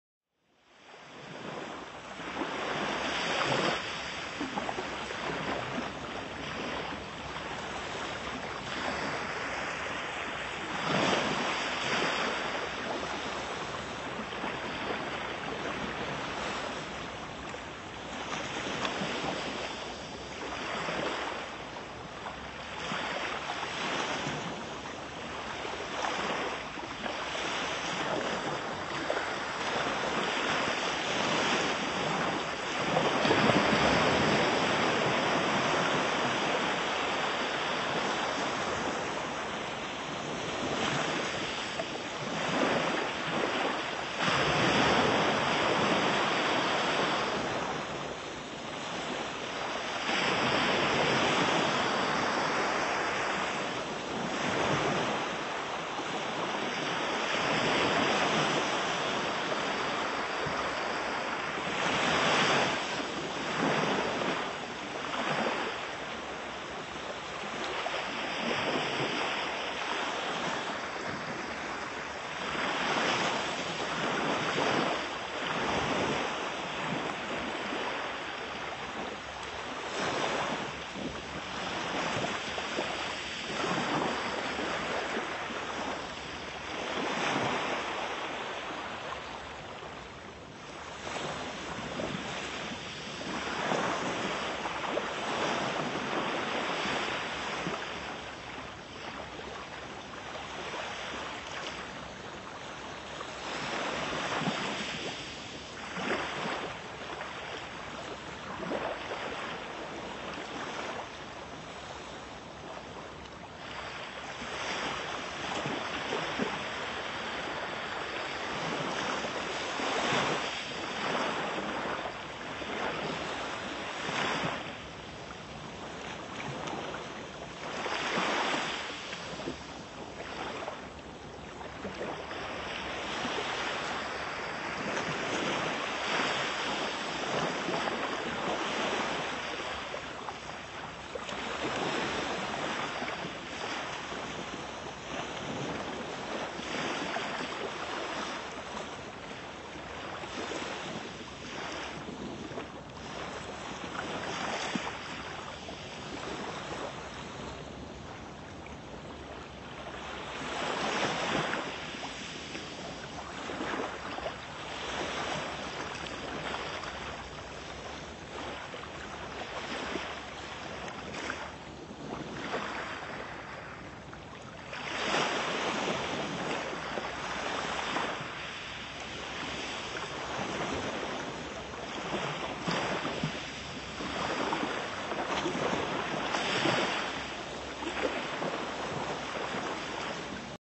Ocean waves music, relaxing music,deep sound, stress release music
Evidence suggests sea noise can help reduce stress and create a calmer atmosphere. The ability of the ocean to bring mental health benefits to people is a growing field of research as scientists increasingly understand the importance of the ocean environment in human health and well-being.